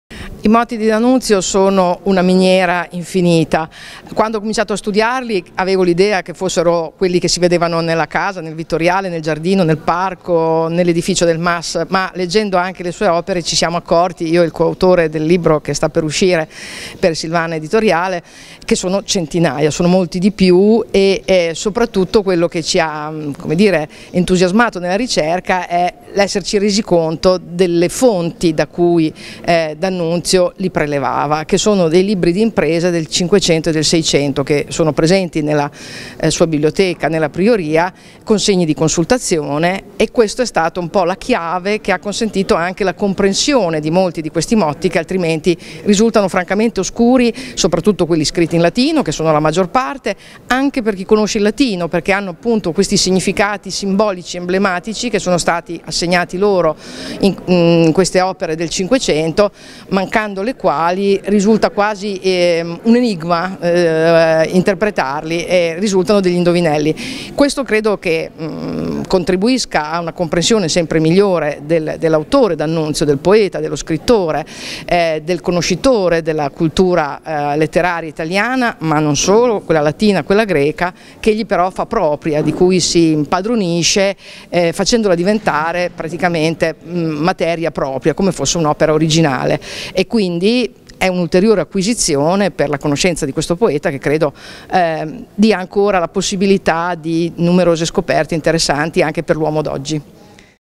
I motti dannunziani secondo Patrizia Paradisi
Alla Festa della Rivoluzione è intervenuta Patrizia Paradisi per parlare dei "motti dannunziani al Vittoriale" in antemprima mondiale